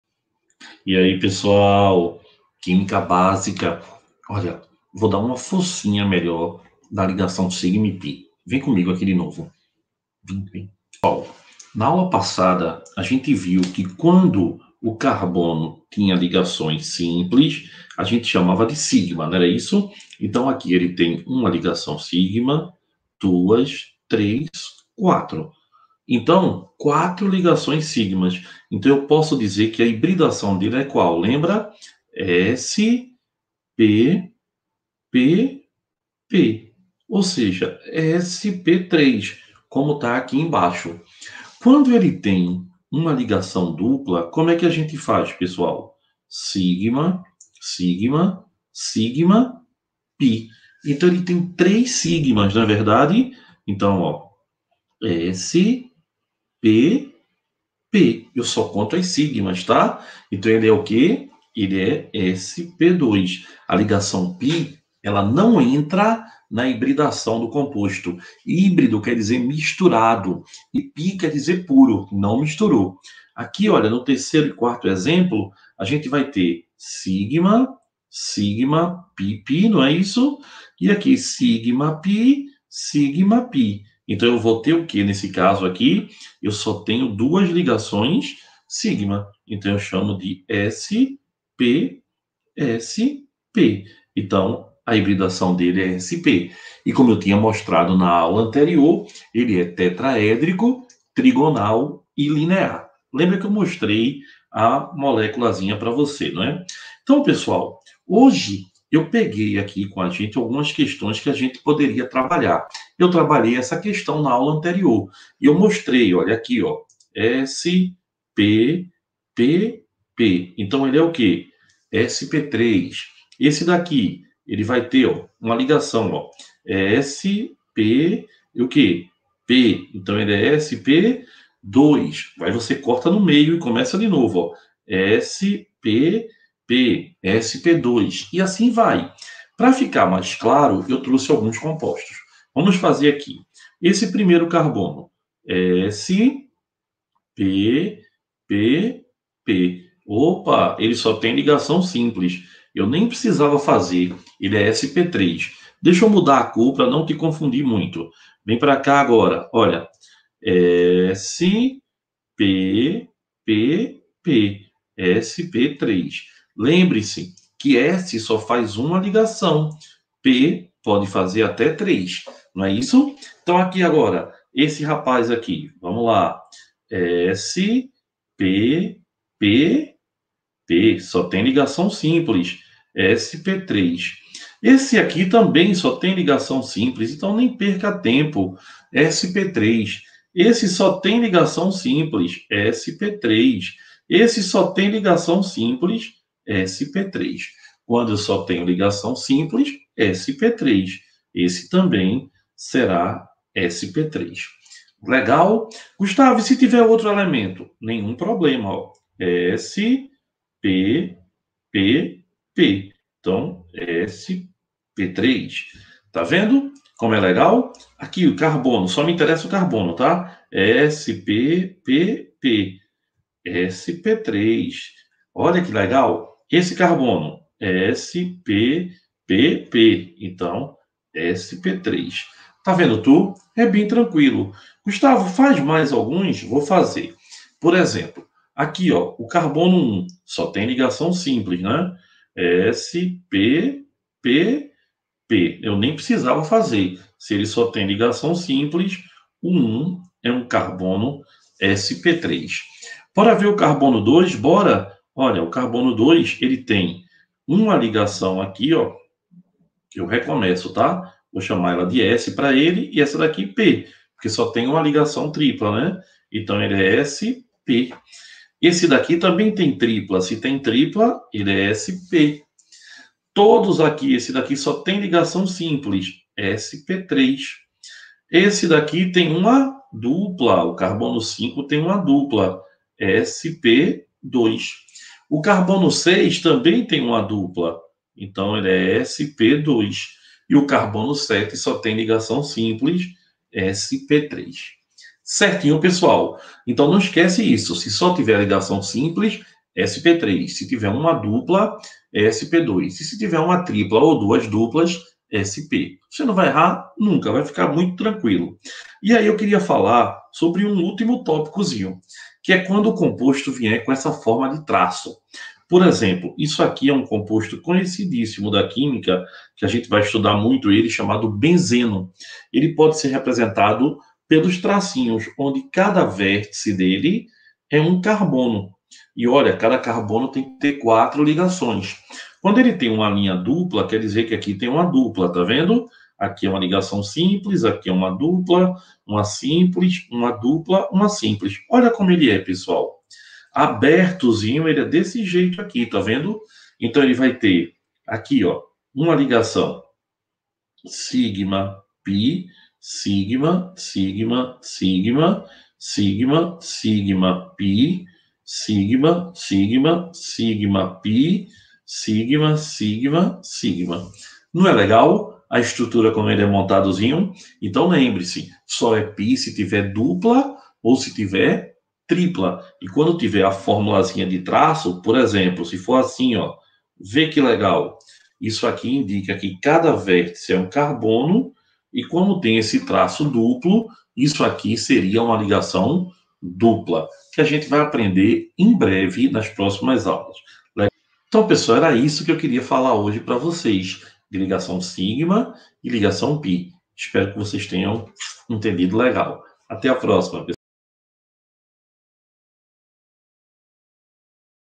AULA 03 - EXERCITANDO A HIBIDAÇÃO
A hibridização do Carbono permite que os átomos desse elemento sejam capazes de realizar quatro ligações químicas, ou seja, o carbono só realiza quatro ligações após sofrer o fenômeno da hibridizaçã